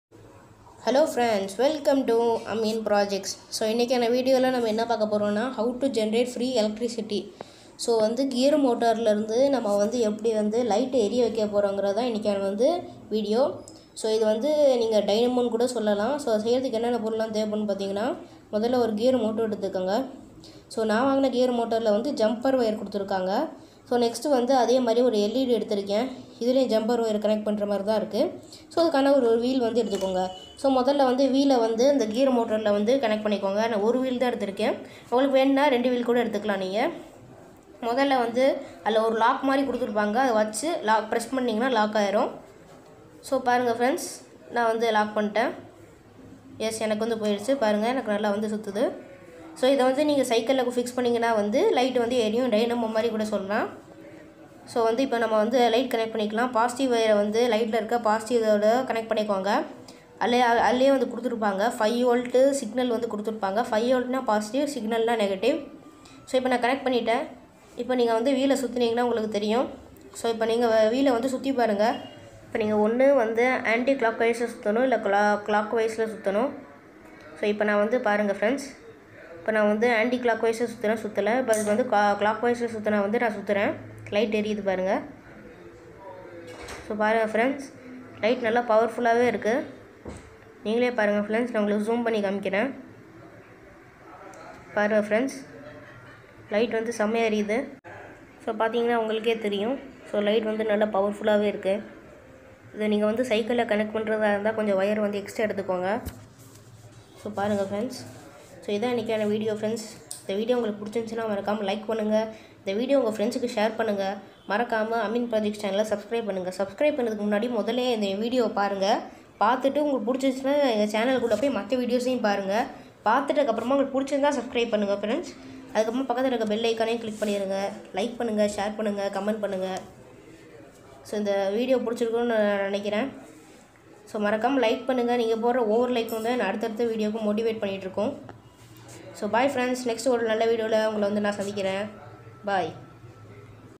How to generate free electricity | 100% working | by Ameen
hello welcome to Ameen projects today in this video I'm going to generate free electricity with motor

The things needed
*Gear motor and it's wheel
*led light

sorry for the delay viewer